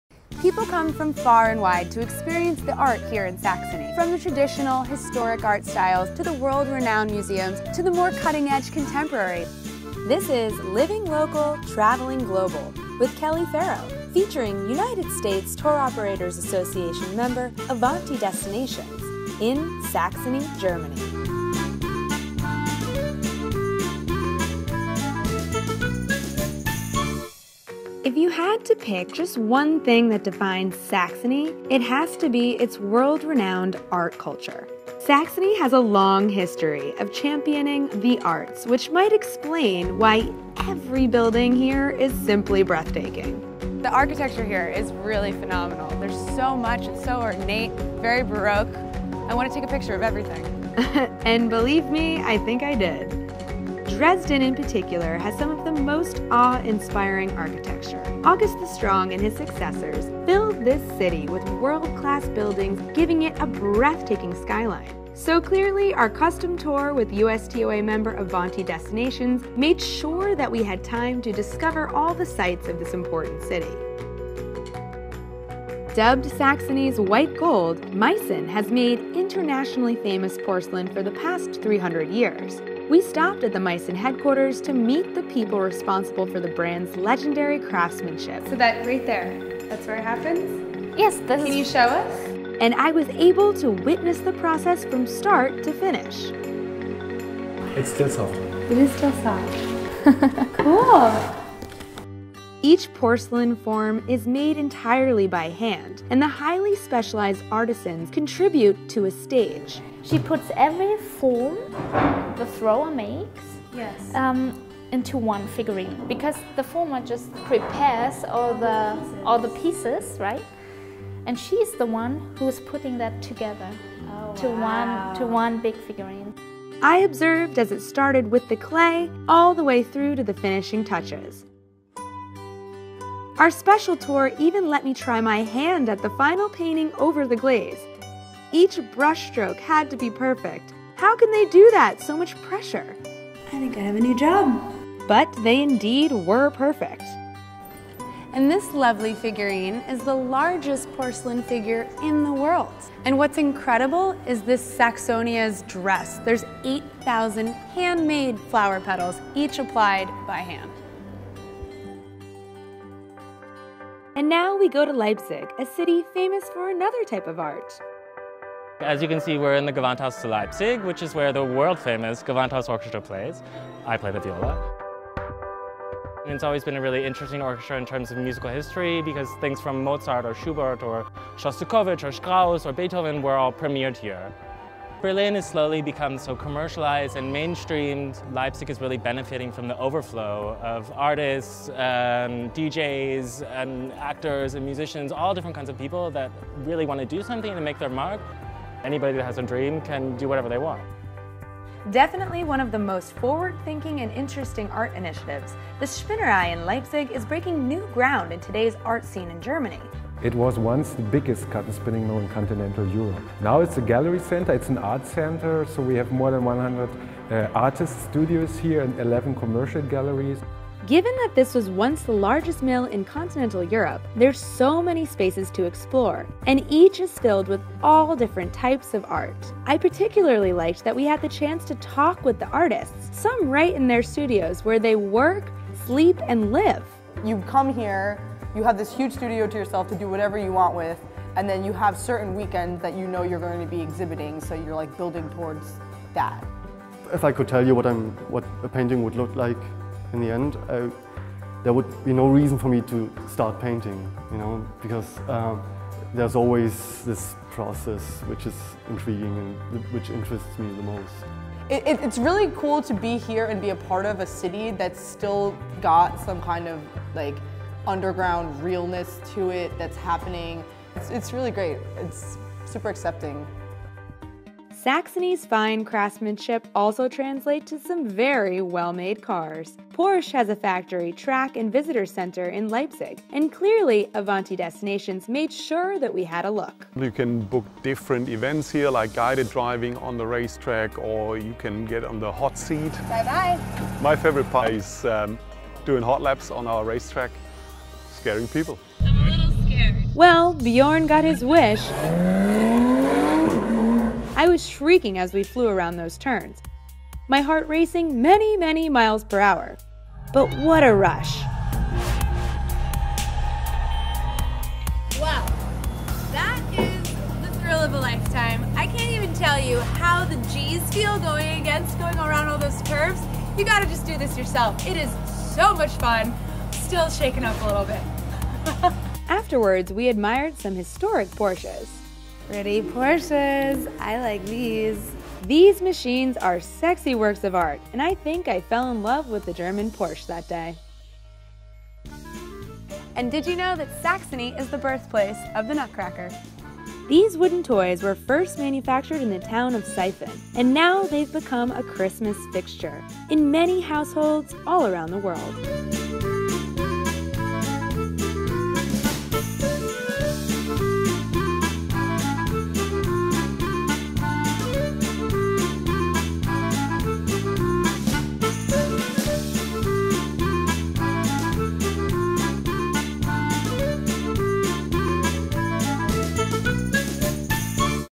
Arts & Culture in Saxony
Kelley Ferro is fascinated by Germany's number one cultural destination.

It is first and foremost the art and cultural events that visitors from all over the world appreciate so much in Saxony. Whether in world-class venues, with famous orchestras and ensembles – internationally noted theatre and musical productions , high-calibre festivals as well as museums and exhibitions represent the Saxon high culture. Welcome to Germany's number one cultural destination.

»Arts & Culture in Saxony« by »Living Local, Traveling Global with Kelley Ferro« in cooperation with United States Tour Operators Association (USTOA) and Avanti Destinations.